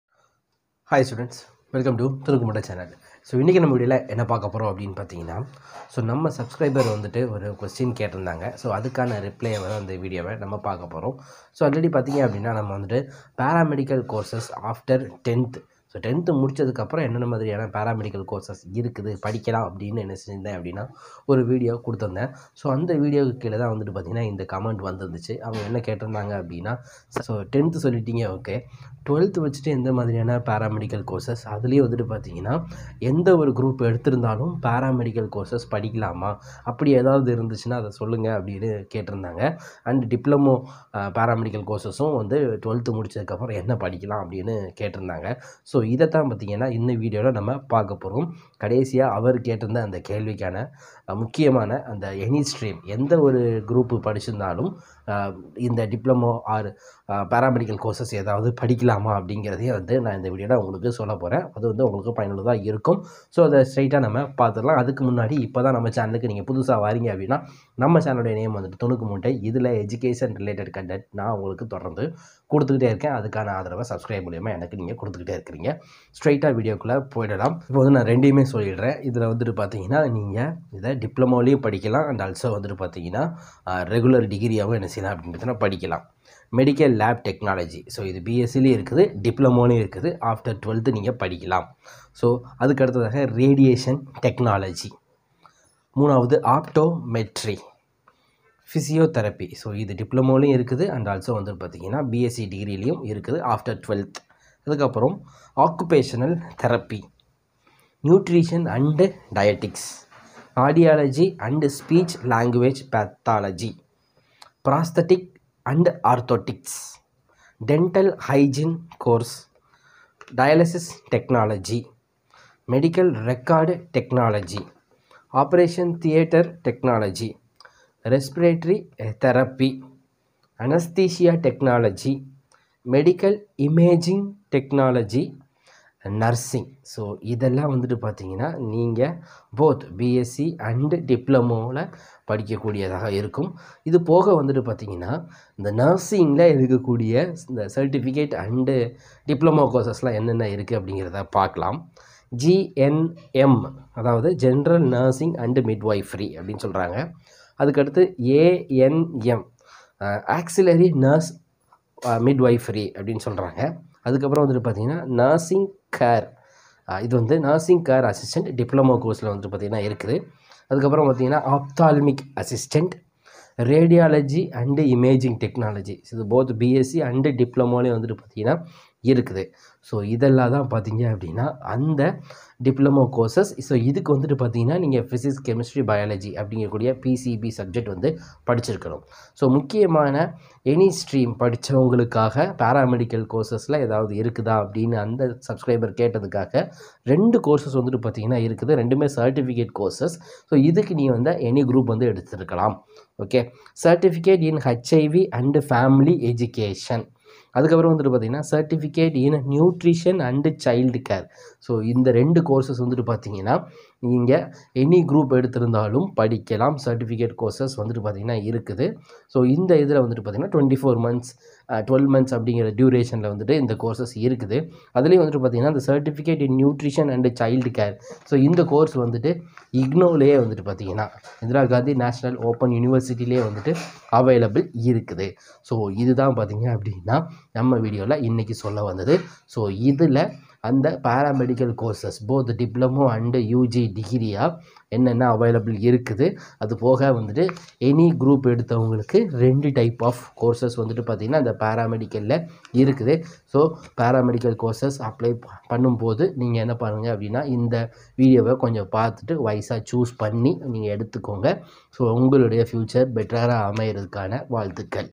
Paramedical Courses After 12th | Any Group 😃
This video gives you details about the paramedicalCourse after 12th Standard any group . It is very useful to the Candidate who search for best diploma courses in Paramedical field.